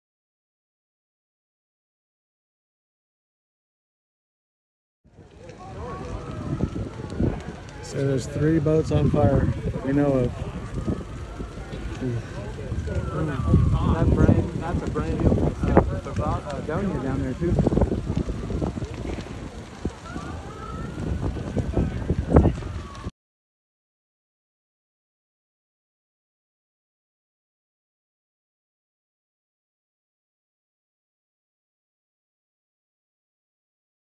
Lake Powell boat fire
What a sad event.

Thankfully no lives were lost.

Final count has 8 boats burned with 3 total loss.